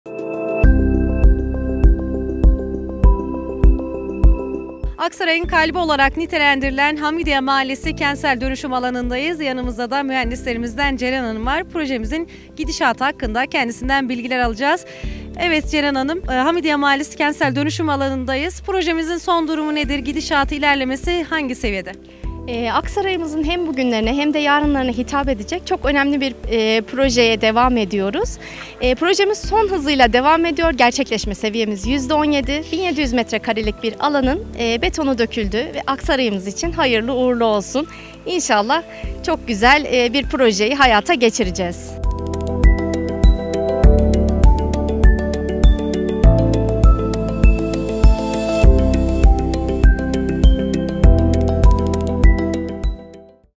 📍Hamidiye Kentsel Dönüşüm Projesi🙋🏻‍♂️
📢 Alandan bildiriyoruz.😎
Depreme dayanıklı 574 birim, çift katlı otopark, 25 mt'lik yeni cadde, modern dükkanlar...